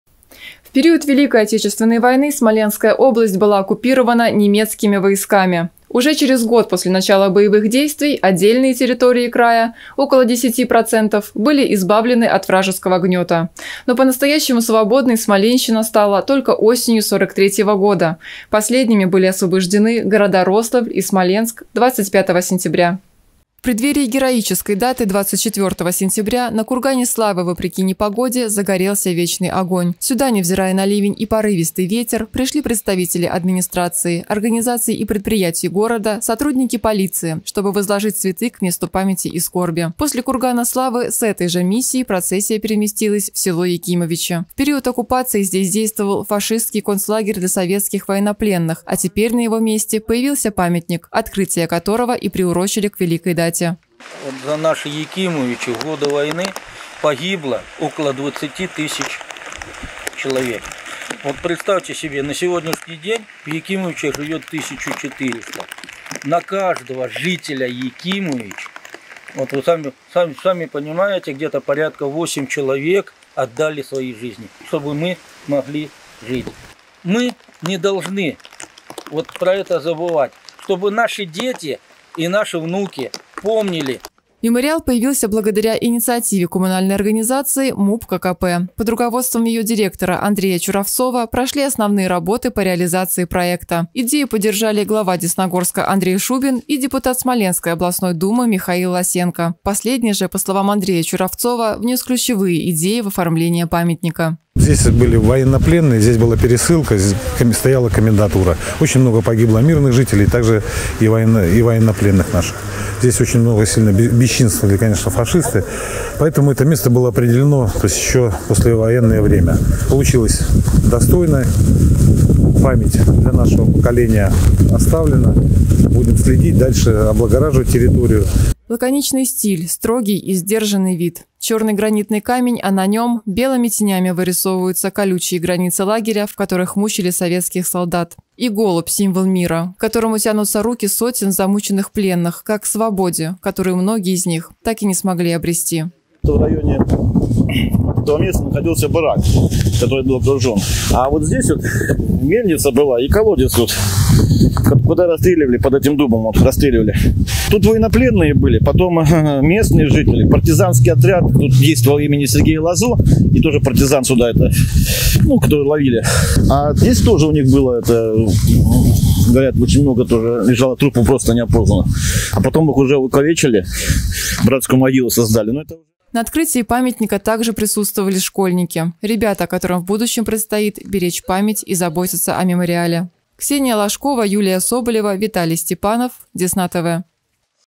Десна-ТВ: Десногорск присоединился к празднованию Дня освобождения Смоленщины
В период Великой Отечественной войны Смоленская область была оккупирована немецкими войсками. Уже через год после начала боевых действий отдельные территории края - около 10% - были избавлены от вражеского гнёта. Но по-настоящему свободной Смоленщина стала только осенью 43 года. Последними были освобождены города Рославль и Смоленск 25 сентября.